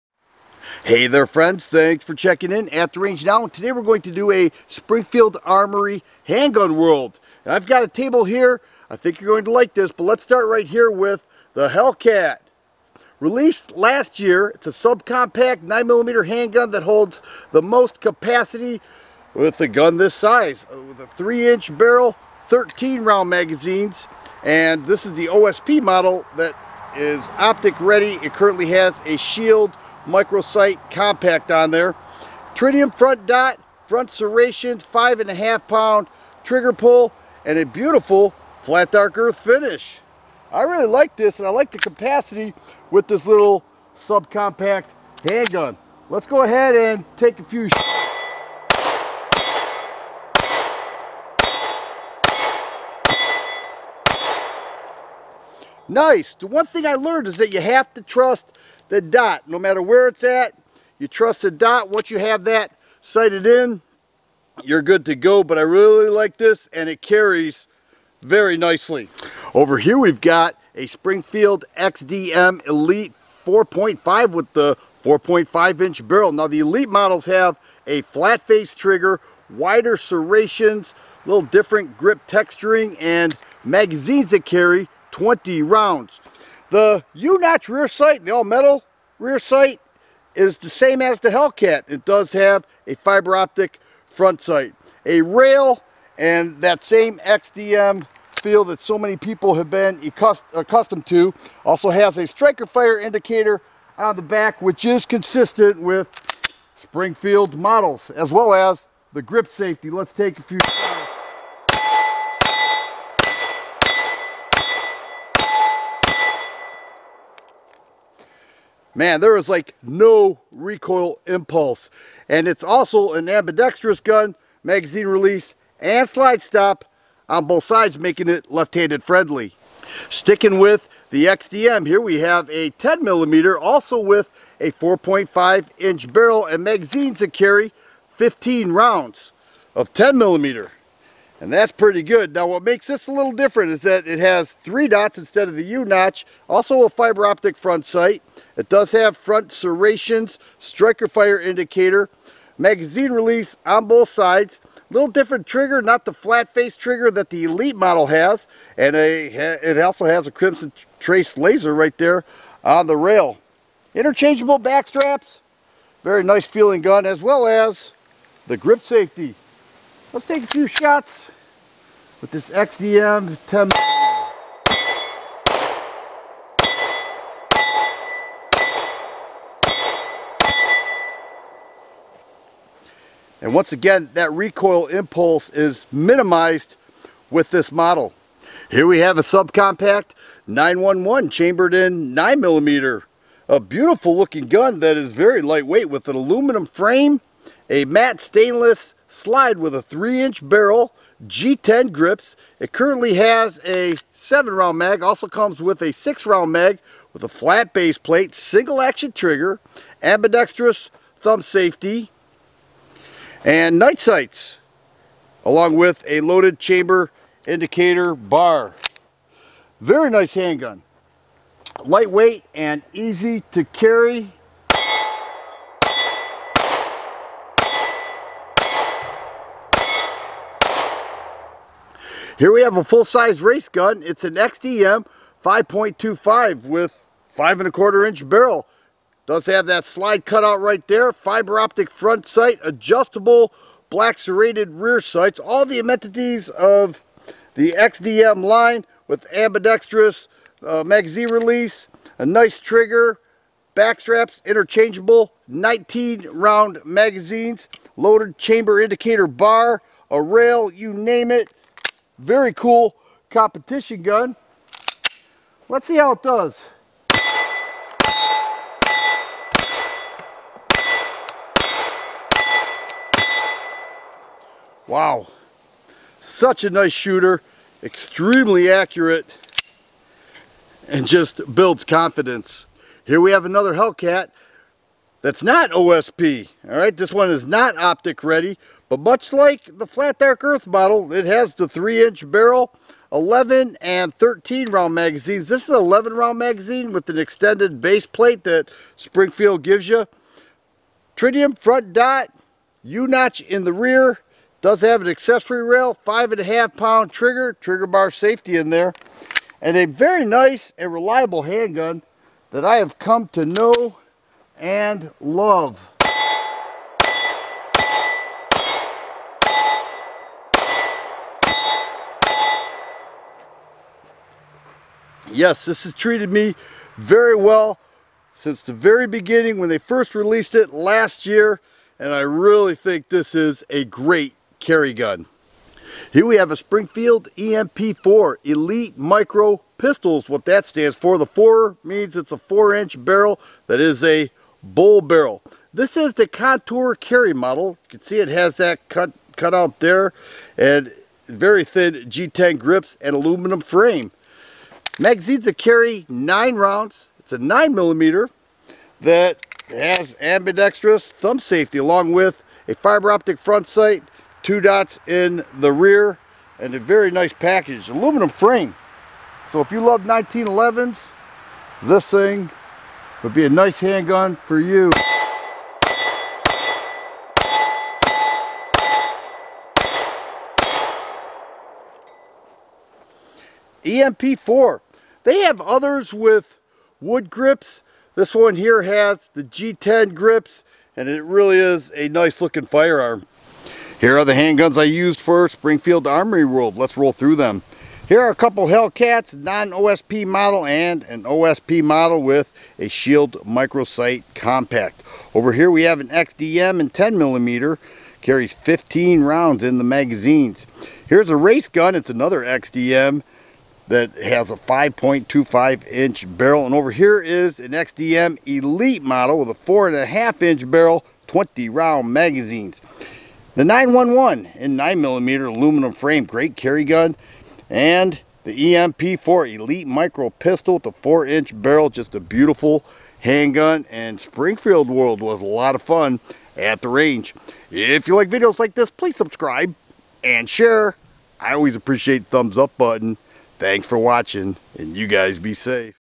Springfield Armory Handgun World (7 Handguns) - TheFirearmGuy
Here is a Springfield Armory "Handgun World" Review with 7 handguns. This is part of my ongoing series of "Handgun World." I featured everything from their subcompact to full size handguns. Let me know which of the handguns featured are your favorites.